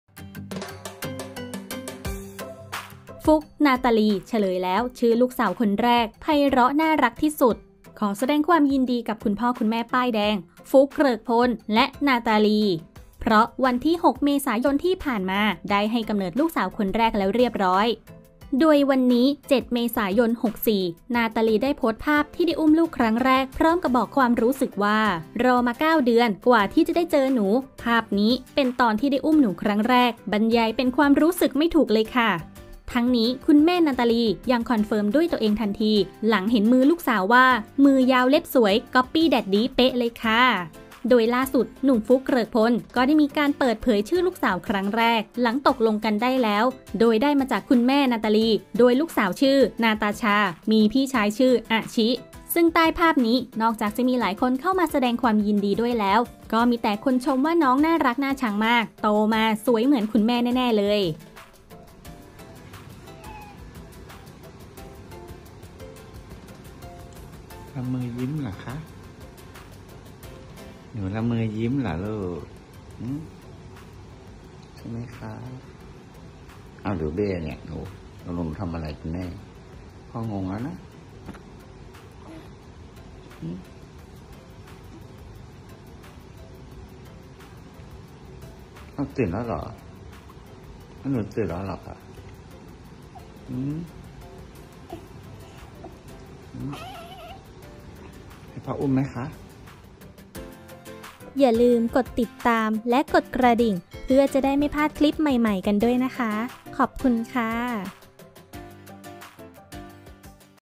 "ฟลุค - นาตาลี" เฉลยแล้วชื่อลูกสาวคนแรก ไพเราะน่ารักที่สุด
ขอแสดงความยินดีกับคุณพ่อคุณแม่ป้ายแดง ฟลุค เกริกพล อีกครั้ง เพราะล่าสุดวันที่ 6 เม.ย. ที่ผ่านมาภรรยาสาว นาตาลี เจียรวนนท์ ได้ให้กำเนิดลูกสาวคนแรกแล้วเรียบร้อย
ทั้งนี้คุณแม่นาตาลี ยังคอนเฟิร์มด้วยตัวเองทันทีหลังเห็นหน้าลูกสาวว่า "มือยาวเล็บสวย copy daddy เป้ะ เลยค่า"

ล่าสุด หนุ่มฟลุค ก็ได้มีการเปิดเผยชื่อลูกสาวครั้งแรก หลังตกลงกันได้แล้ว โดยได้มาจากคุณแม่นาตาลี ลูกสาวชื่อ  'นาตาชา' มีพี่ชายชื่อ 'อชิ'
" Hello ka . My name is Natasha Daughter of mommy Natalie & Daddy Fluke ka Taken on the 2nd day of my life BabyNataShaOfFlukeLee"